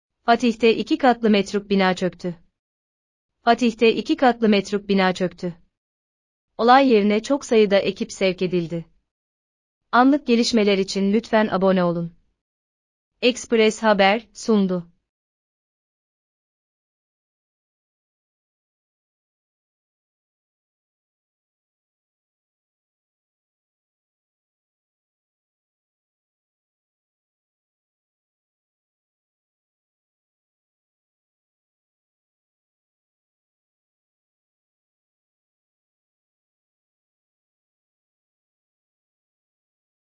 Fatih’te 2 katlı metruk bina çöktü
Fatih’te 2 katlı metruk bina çöktü. Olay yerine çok sayıda ekip sevk edildi.